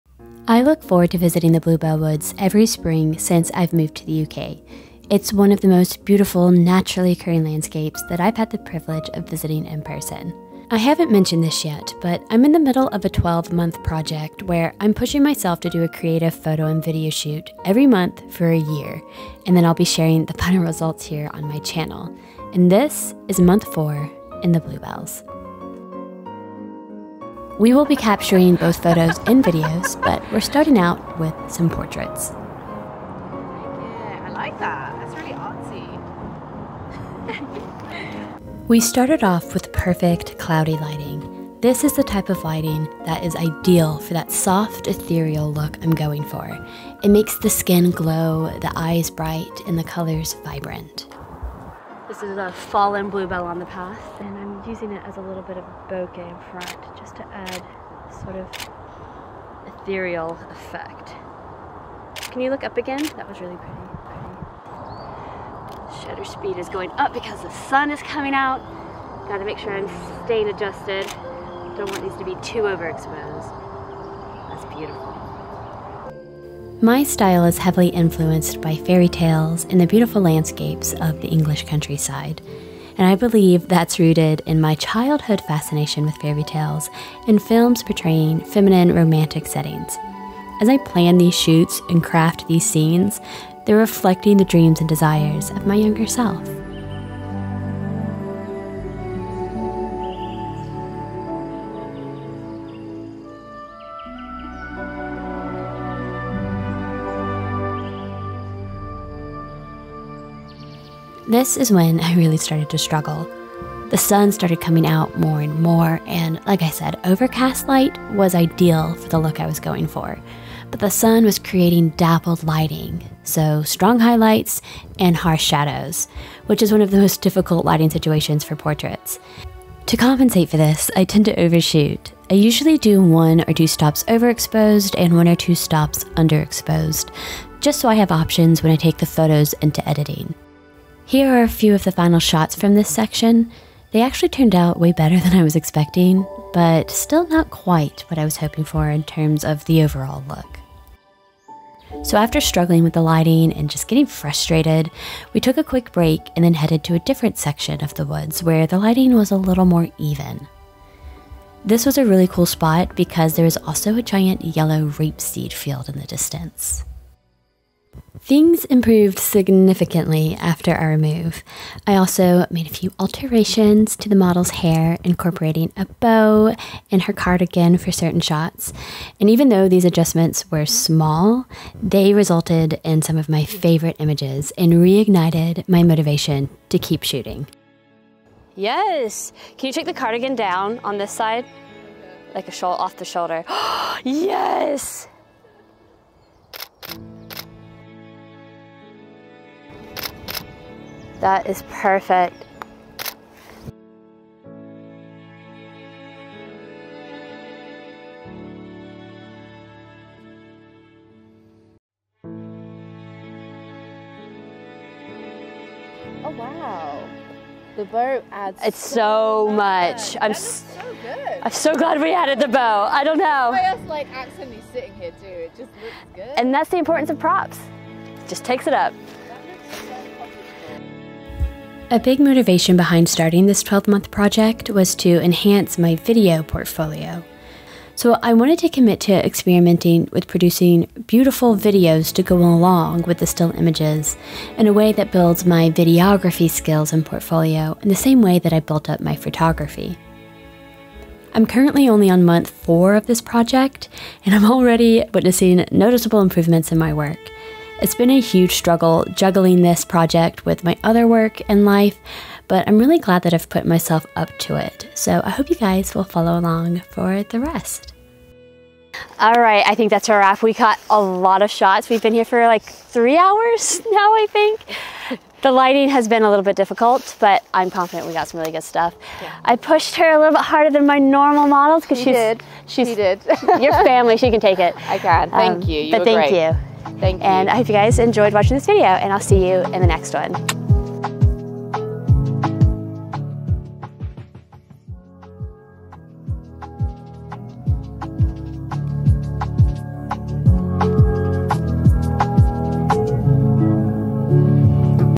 Enchanting Bluebell Photo & Video Shoot | Month 4
This is month 4 in my 12 month creative shoot project! This time we are taking some magical portraits and videos amongst the beautiful bluebells. My model, and sister-in-law, Helena, brings a cottagecore/Alice in Wonderland styled look.